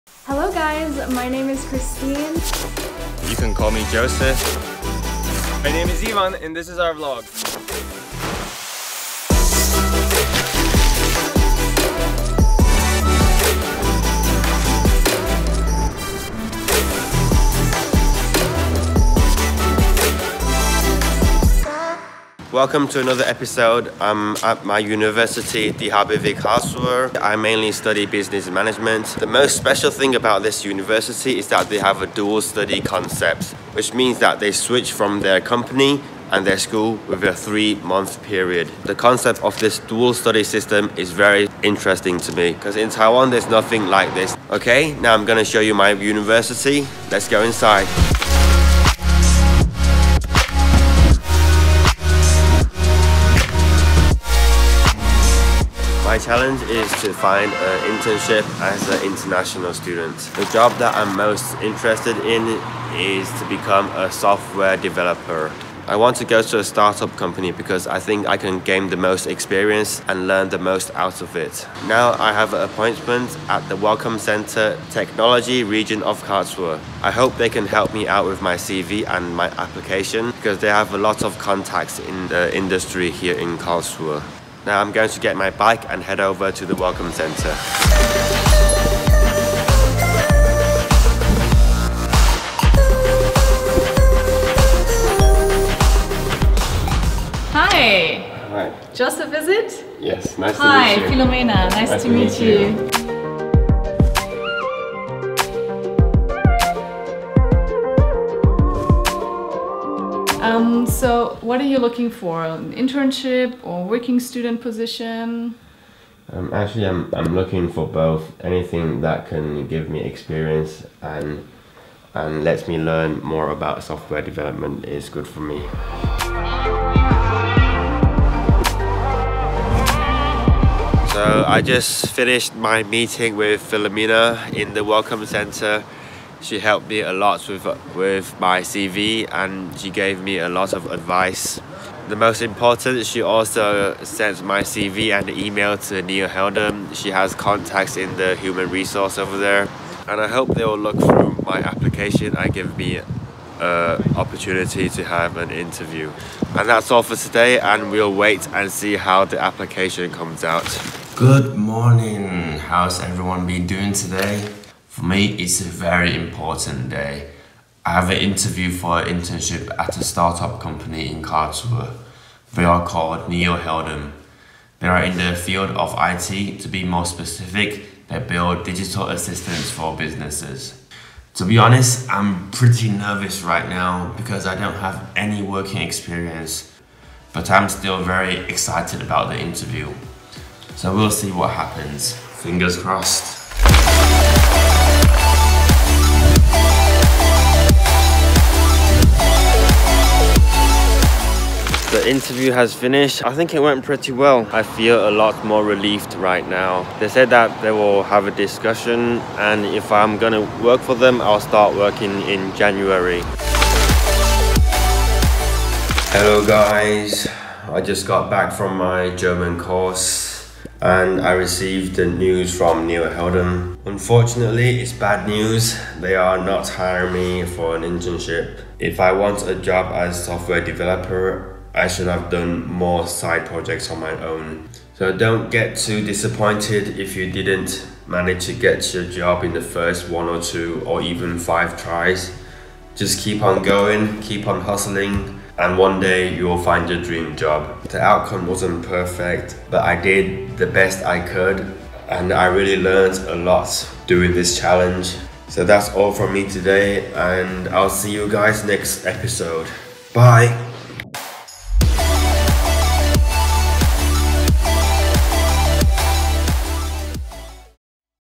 Finding an Internship in Germany | International Student Vlog 2019/20
Joseph in on the look out for an internship at a software development company to gain work experience. In this video, he takes us along to two interviews. Will he get the job?